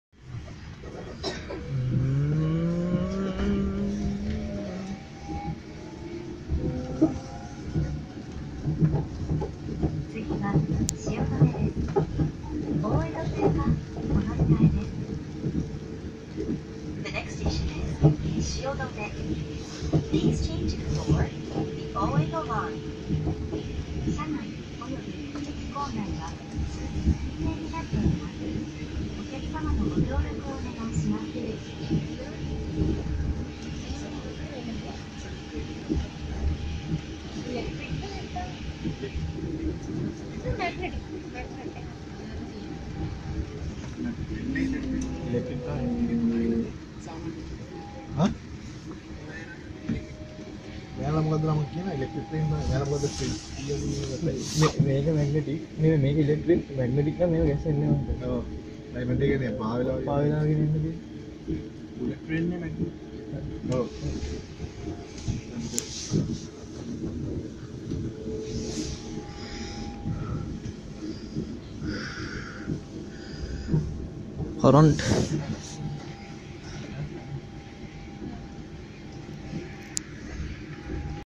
Japan Electric Train
The railway system in Japan has a high reputation for punctuality and safety.

The train fare varies naturally depending on the distance you travel as well as the type of train you wish to catch: Limited Express, Express etc. and the type of reserved seat: Green Car (first class), Sleeper etc., for each of which an extra charge is required.

Tickets for short distances are available from ticket machines that are installed at each train station whereas tickets for long distances and reservations are dealt with at ticket offices at major stations.